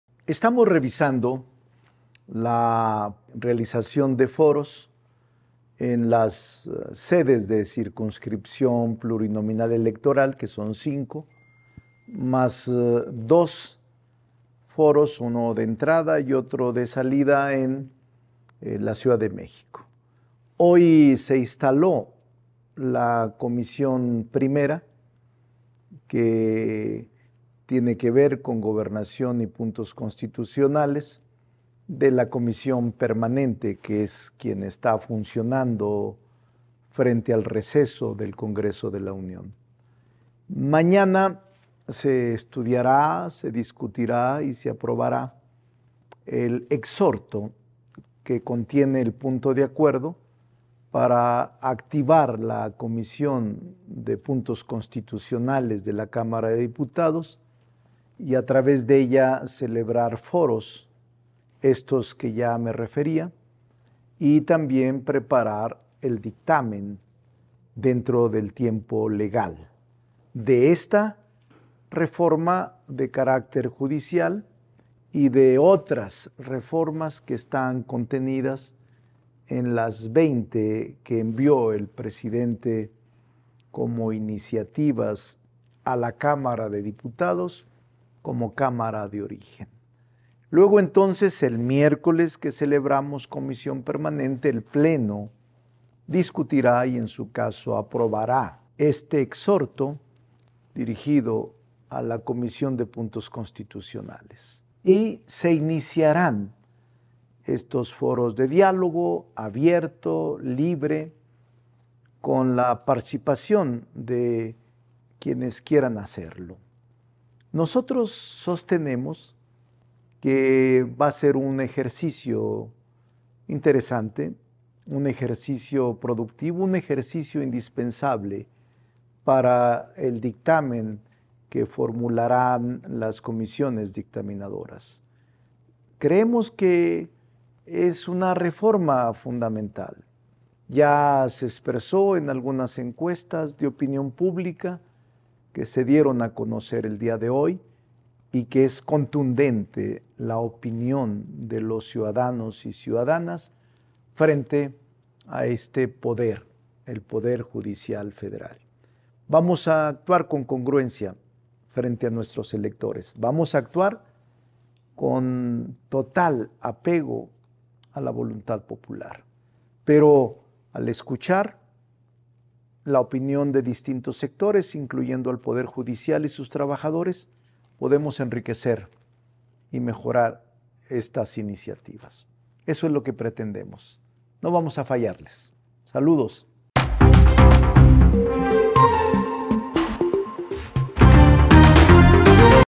Se acerca el eminente final del corrupto Poder Judicial: Ricardo Monreal
Se confirma la necesidad de reformar al Poder Judicial. En el Congreso de la Unión actuaremos con congruencia y total apego al mandato del pueblo de México.